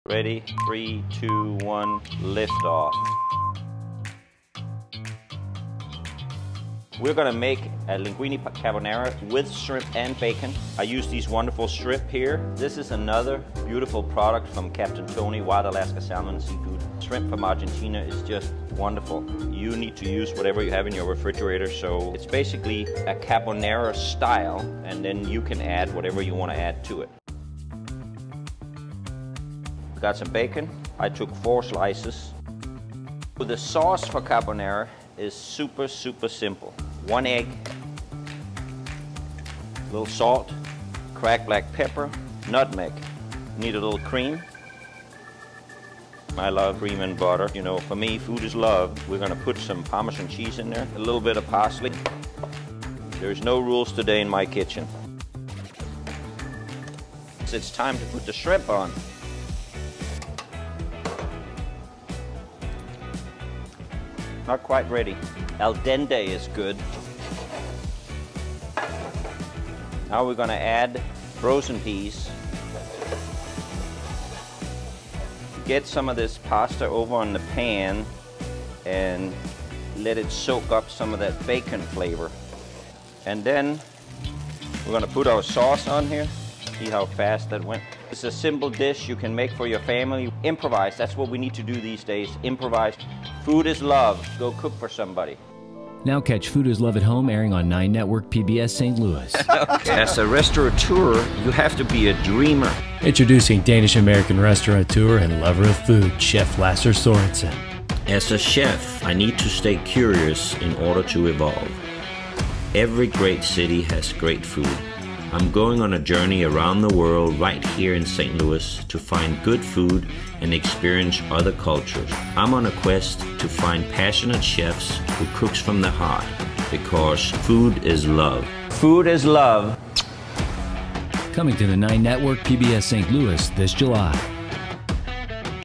Easy Recipe for Linguine Carbonara with Bacon and Shrimp
In this episode of Food is Love at Home I demonstrate how quickly you can improvise Linguine using Bacon & Shrimp